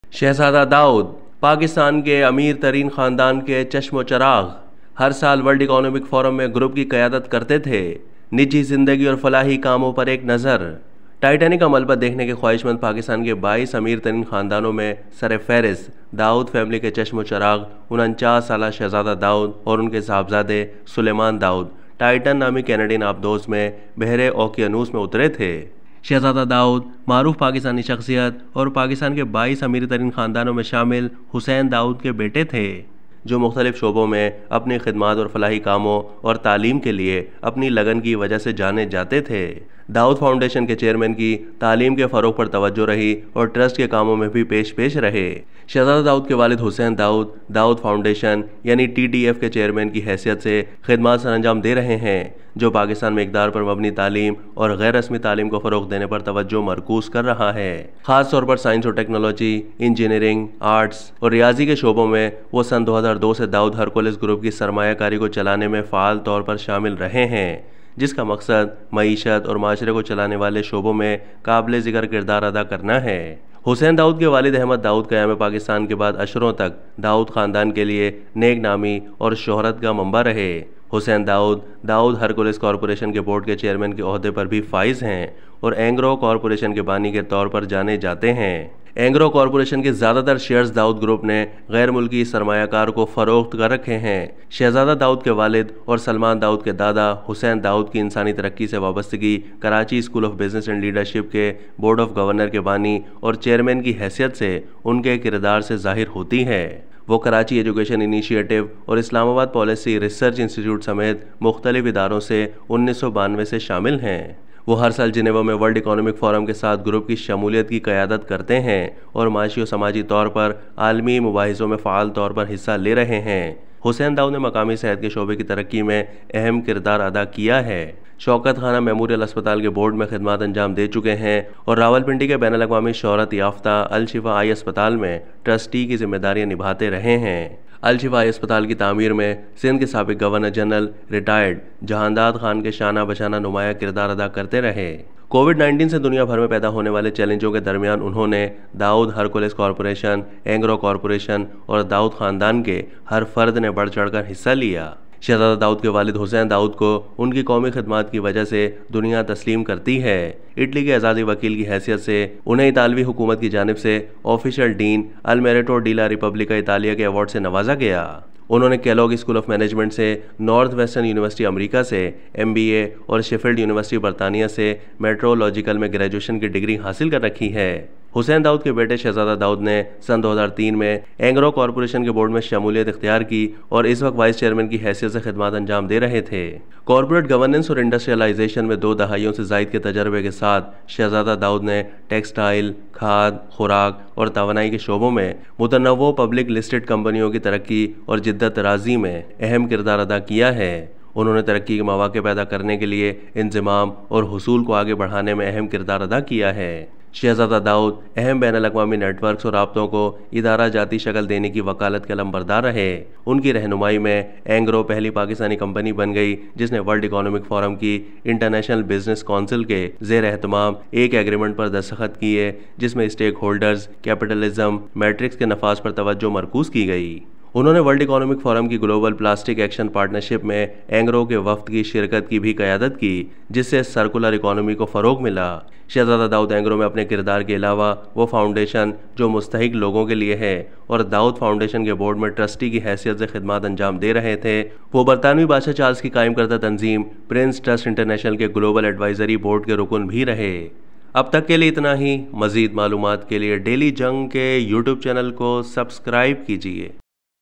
Shahzada Dawood used to lead the group every year at World Economic Forum
Shahzada Dawood is the son of Hussain Dawood, a well-known Pakistani personality and one of the 12 richest families in Pakistan